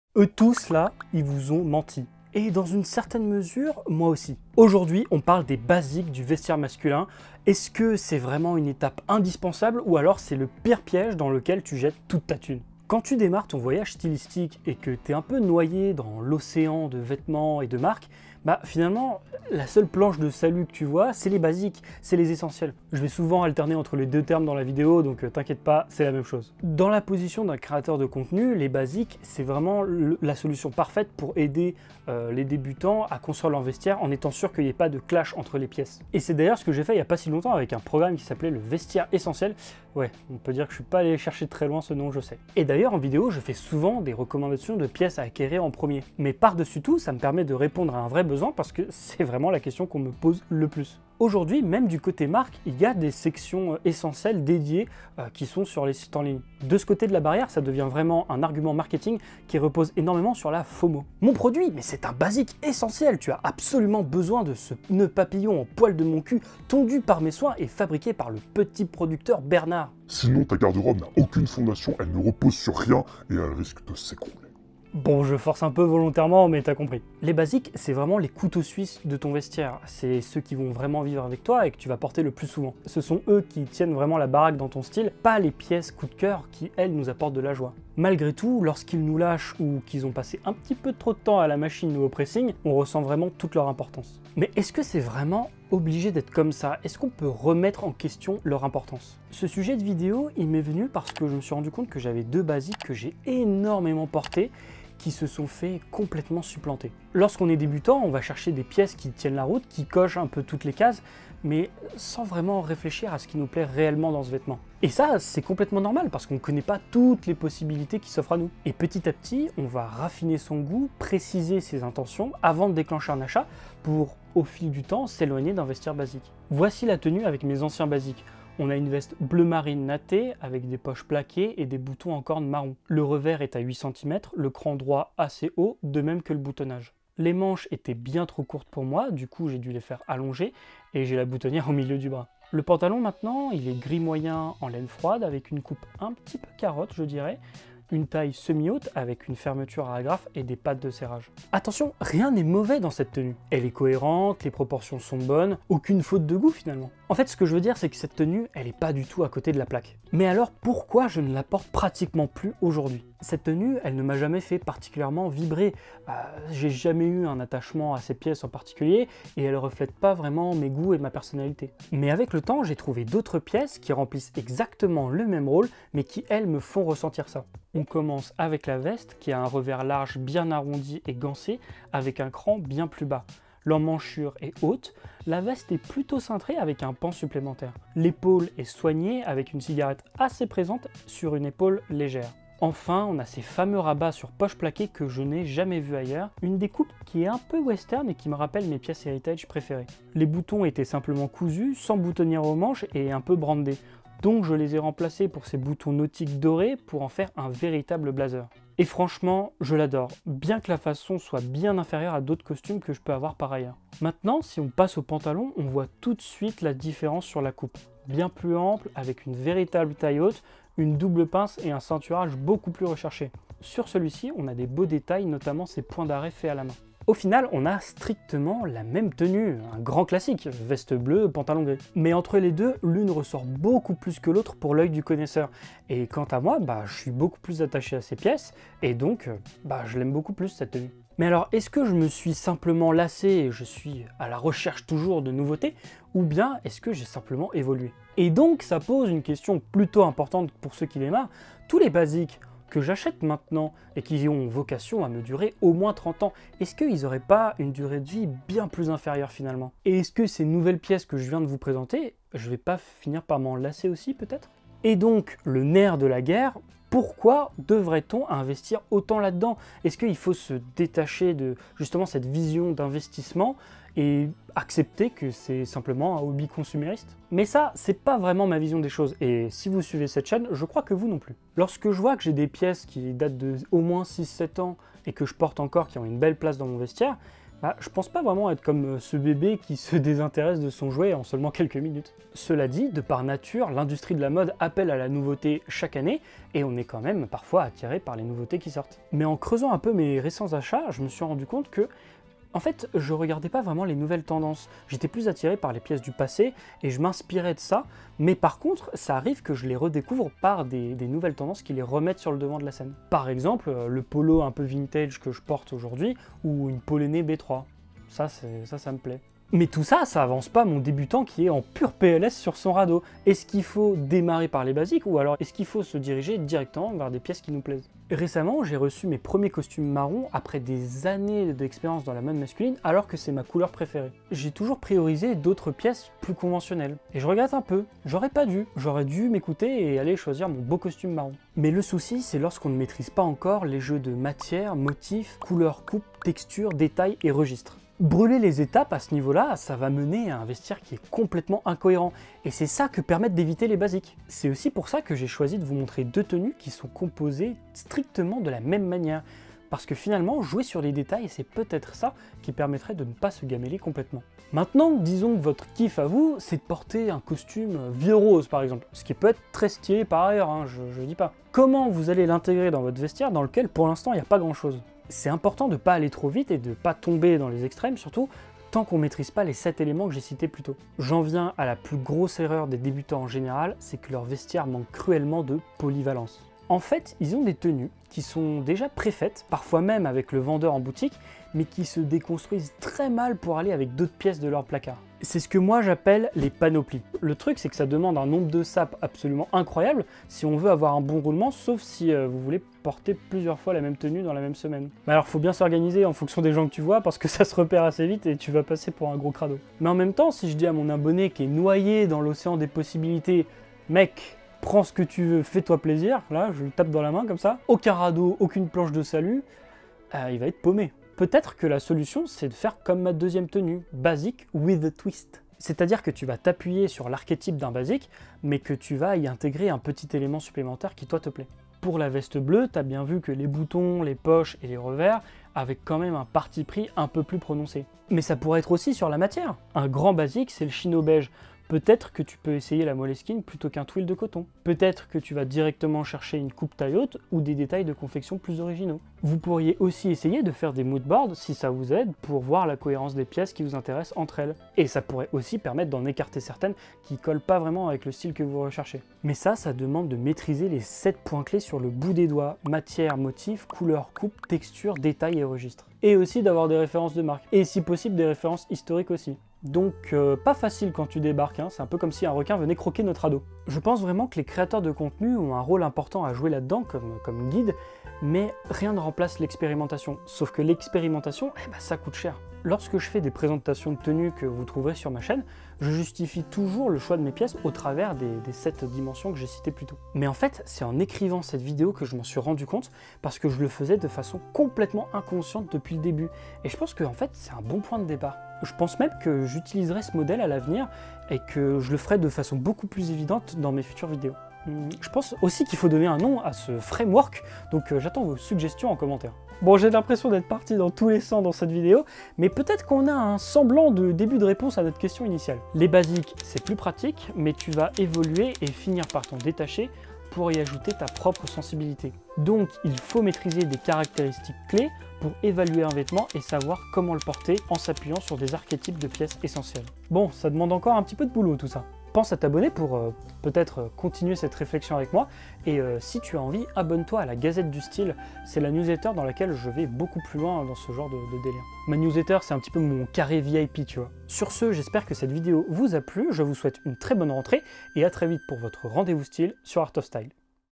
As-tu VRAIMENT BESOIN des BASIQUES ? Art of Style
✏️ As-tu vraiment besoin des basiques dans ton vestiaire ?
On en parle partout, le contenu pullule sur toutes les plateformes sur ce sujet.

Présentés comme absolument essentiels, ils suscitent la FOMO et les marques en profitent pour proposer des pièces simples qui se vendent facilement

Mais est-ce vraiment le cas ?

Aujourd'hui on essaye de répondre à cette question, avec une expérience personnelle à l'appui et un début de réponse en fin de vidéo !

📖 Sommaire
Le problème des basiques: 0:00
Anciens basiques vs nouveaux: 2:05
Comment faire quand on débute ?: 5:20
Ma réponse aux basiques: 7:00